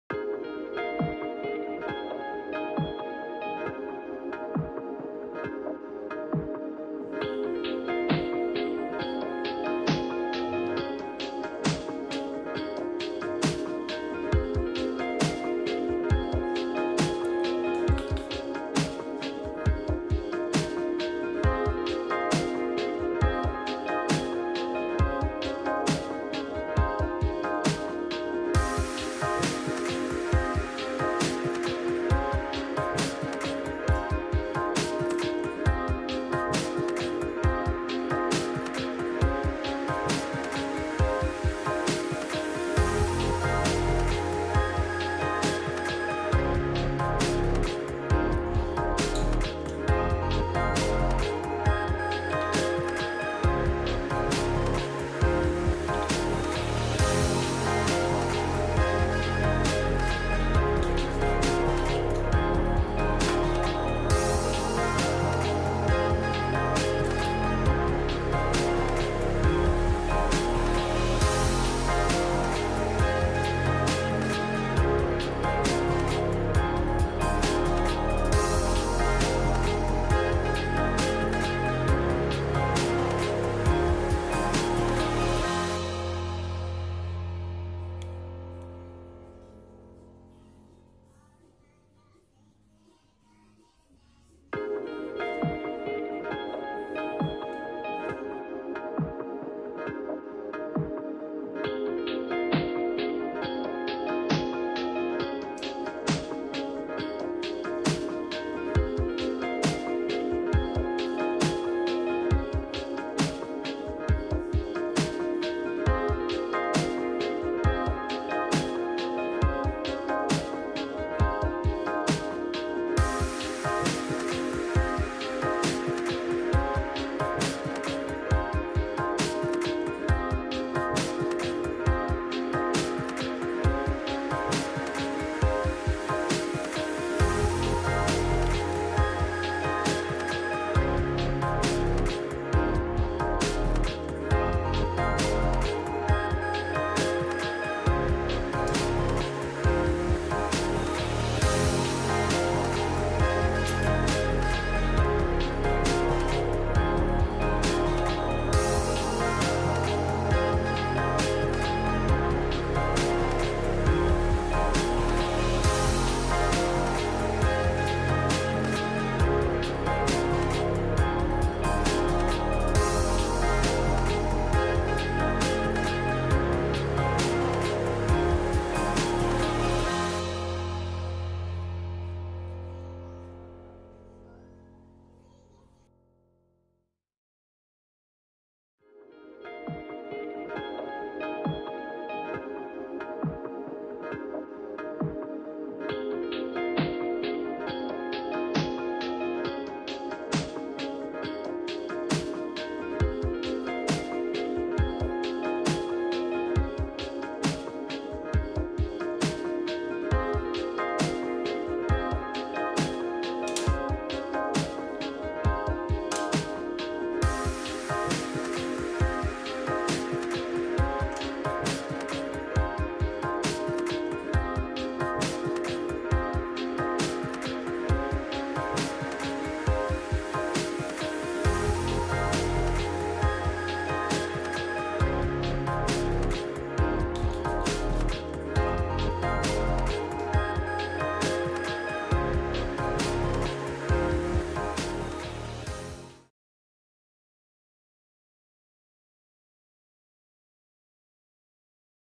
Make-up routine Melissa Met Jane Iredale en Revitalash
Wat is mijn favoriete Makeup voor dagelijks.
Makkelijk en mooi resultaat.